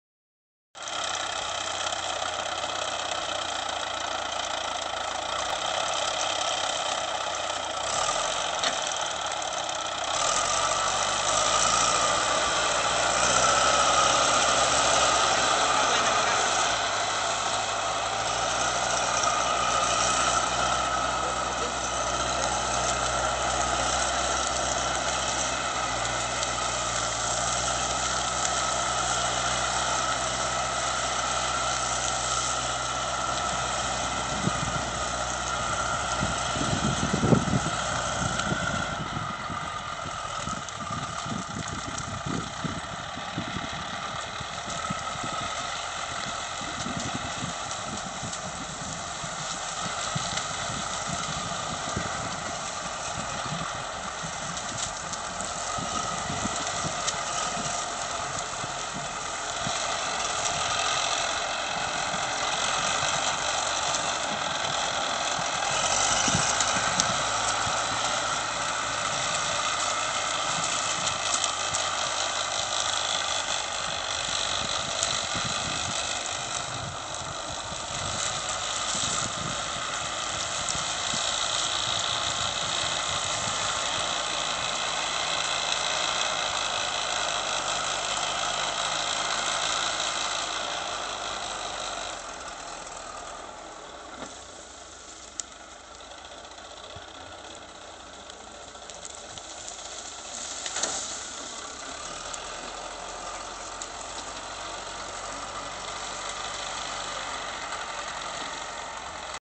Acopio palma CAT 944 Grupo FBG 2
Acopio de troncos de palma para alimentar al Bandit chipper 3680, proyecto de Reciclaje de Nutrientes de Palma de aceite de la empresa Agropalma S.A, ejecutado por Grupo Biotecnológico FBG de Colombia SAS. Grupo FBG SAS.